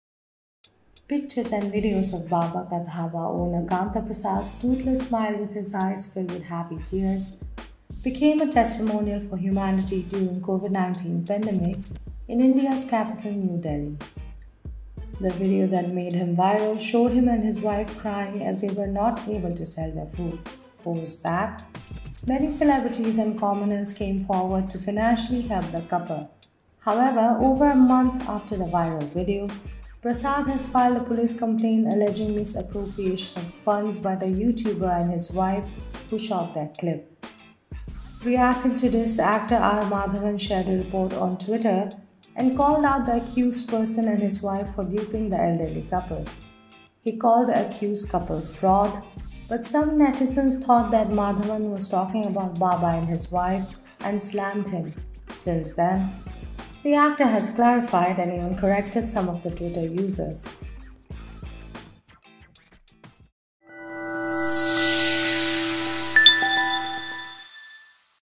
Netizens think R Madhavan disrespects 'Baba Ka Dhaba' couple, actor clarifies on 'fraud' remark
Pictures and videos of 'Baba Ka Dhaba' owner Kanta Prasad's toothless smile with his eyes filled with happy tears became a testimonial for humanity during COVID-19 pandemic in India's capital, Delhi. The video that made him viral showed him and his wife crying as they were not able to sell their food. Post that many celebrities and commoners came forward to financially help the couple. However, over a month after the viral video, Prasad has filed a police complaint alleging misappropriation of funds by the Youtuber and his wife, who shot their clip. Reacting to this, actor R Madhavan shared a report on Twitter and called out the accused person and his wife for duping the elderly couple. He called the accused couple 'fraud' but some netizens thought that Madhavan was talking about Baba and his wife and slammed him. Since then, the actor has clarified and even corrected some of the Twitter users.